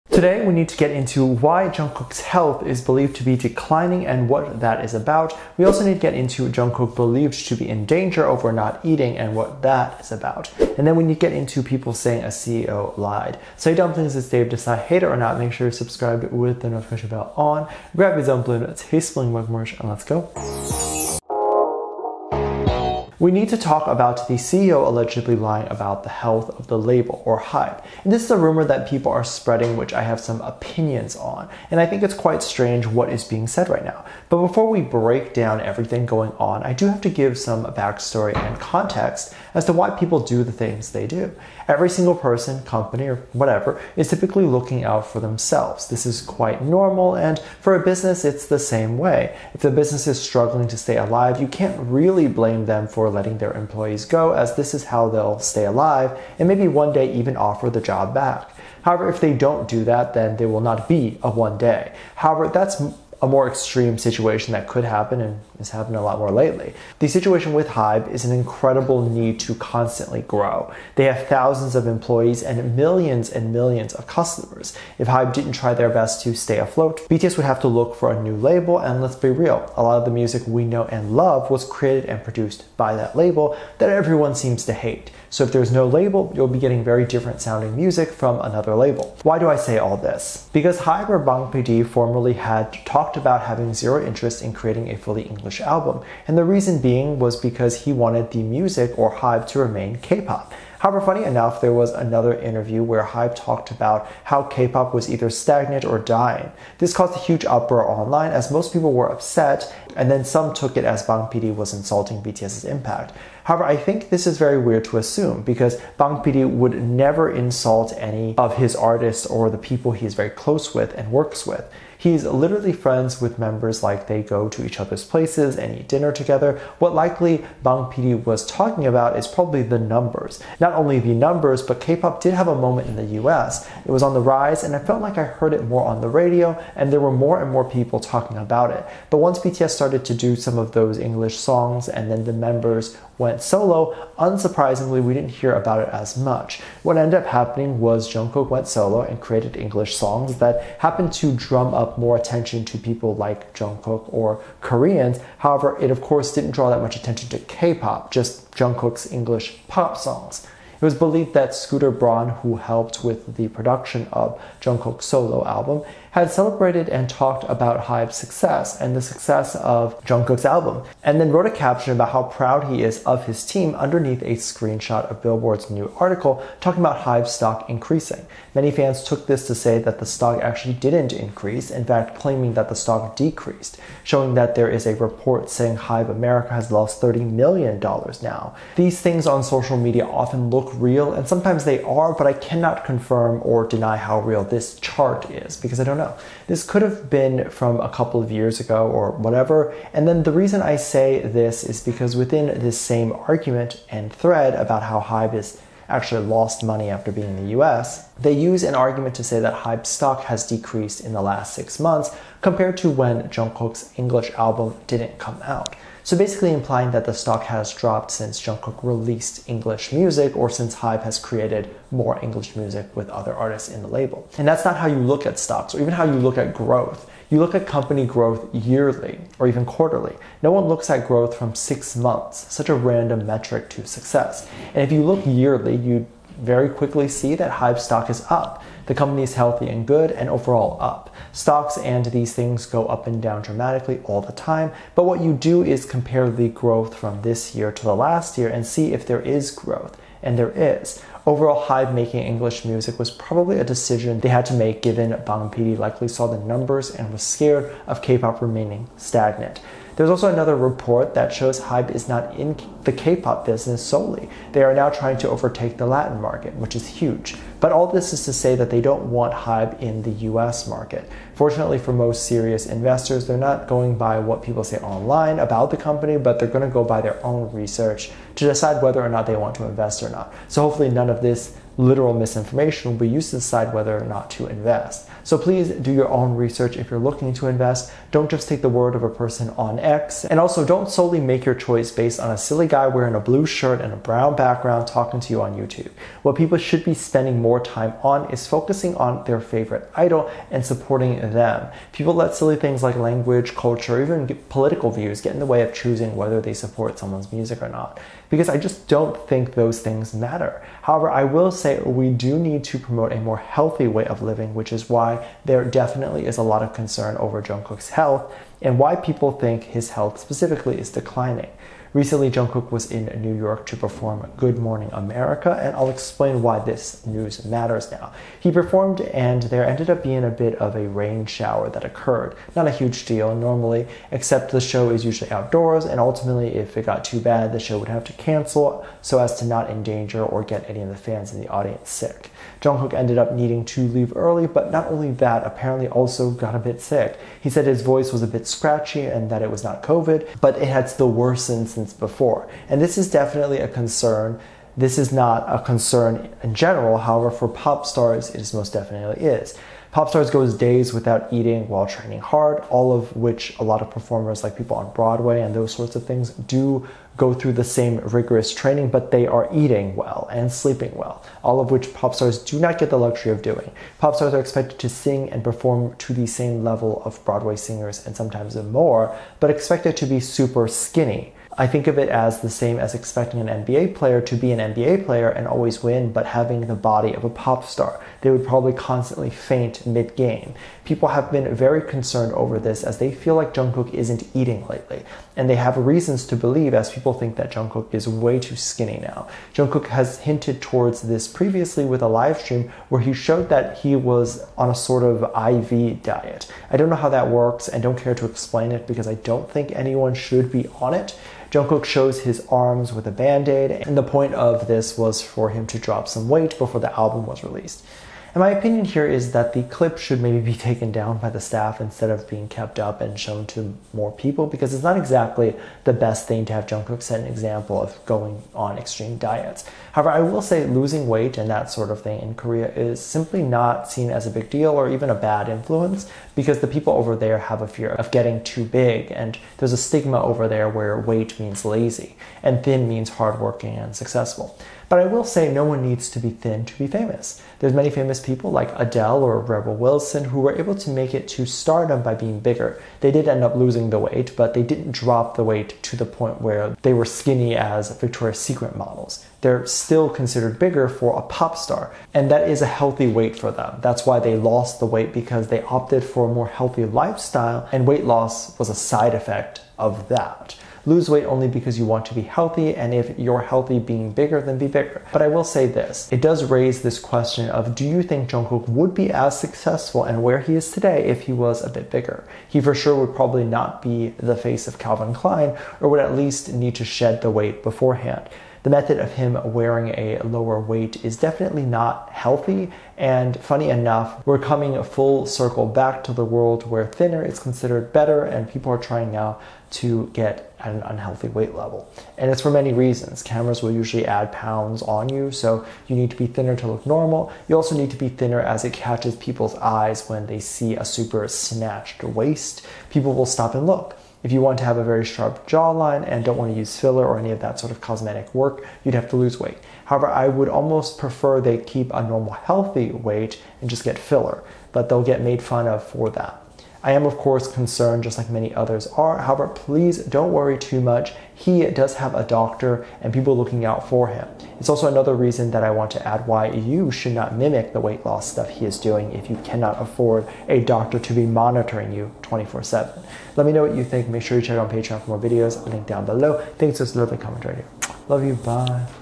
Jung Kook Admits Health Decline! Staff Posts JK In DANGER After NOT Eating? CEO LIES Over Health!
Hey Dumplings, toay I want to get into more KPop news and specifically BTS news. I want to talk about Jung Kook's health declining during an event and what happened. I want to get into staff posting Jung Kook might be in danger after not eating and what happened there. Then we need to get into CEO lying about the health of their company and that rumor.

0:00 Table of Contents
0:23 CEO Lies About Health of HYBE Rumor
4:00 Jung Kook Health Declines After events
6:00 Jung Kook Stops Eating Concerns
9:03 What You Can Do Next To Support

Learn Korean!